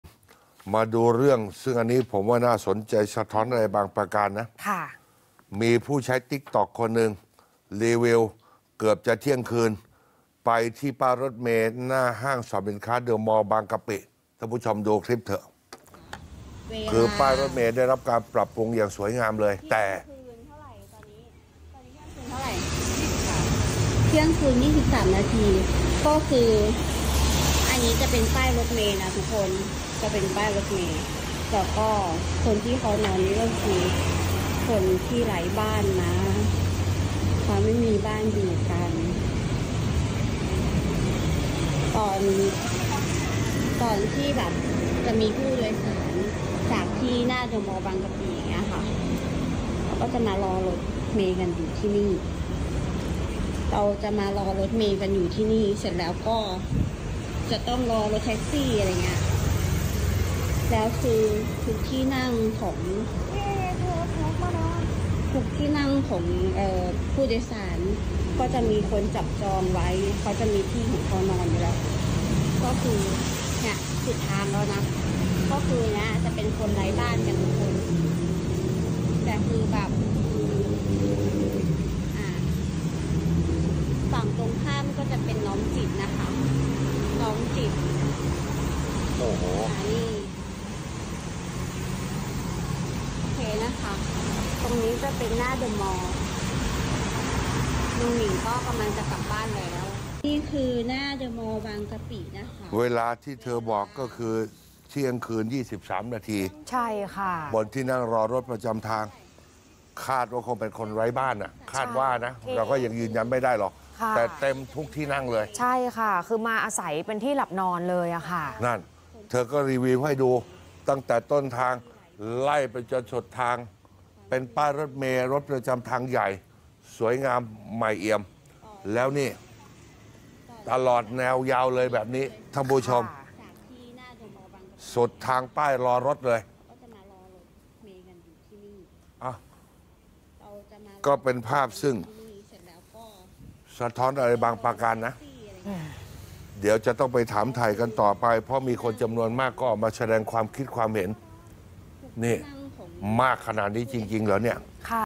วอนจัดระเบียบ! สาวรีวิว ป้ายรถเมล์หน้าห้างดังบางกะปิ น่าใช้ แต่คนไร้บ้านยึดเป็นที่นอน
สาวอัดคลิปรีวิว ป้ายรถเมล์หน้าห้างดังบางกะปิ ทำใหม่ น่าใช้งาน แต่คนไร้บ้านยึดเป็นที่นอน เต็มทุกที่นั่ง จนไม่มีที่นั่งรอรถ หวั่นอันตรายช่วงกลางคืน ชาวบ้านวอนจัดระเบียบ

วานนี้ 29 ก.ค. 67 มีสมาชิก TikTok @jeeraporn.ning โพสต์คลิปรีวิวป้ายรถประจำทางหน้าห้างสรรพสินค้าเดอะมอลล์บางกะปิ หลังเธอกำลังมายืนรอรถที่ป้ายรถประจำทางดังกล่าว โดยระบุว่า “คนไร้บ้าน หน้าเดอะมอลล์บางกะปิ”

โดยในคลิป เป็นป้ายรอรถประจำทาง ซึ่งถูกสร้างขึ้นมาใหม่ สะอาด กว้างขวาง และมีที่นั่งรอรถแบบสบาย ไม่ว่าจะมารอรถประจำทาง หรือรถโดยสารสาธารณะ หรือแม้กระทั่งรอเพื่อน ญาติ

แต่ทว่าบนที่นั่งรอรถประจำทางดังกล่าว กลับถูกจับจองไปด้วยคนไร้บ้าน ที่ใช้อาศัยเป็นที่หลับนอนส่วนตัว ซึ่งเธอได้พูดในคลิปตอนที่เดินมาถึงว่า ขณะนี้เป็นเวลา 24.23 นาที บนที่นั่งรอรถประจำทางกลับมีคนไร้บ้านมานอนจนไม่มีที่นั่ง สุดท้ายเธอต้องไปยืนรออยู่สุดป้ายรอรถ

หลังโพสต์ดังกล่าวเผยแพร่ไป ก็มีคนเข้ามาแสดงความคิดเห็นจำนวนมาก โดยพูดถึงเกี่ยวกับคนไร้บ้าน และยังมีคนมาเล่าประสบการณ์ที่เคยนอนที่ป้ายรอรถประจำทางนี้อีกด้วย รวมทั้งอยากให้ผู้ที่มีส่วนเกี่ยวข้องมาจัดระเบียบด้วย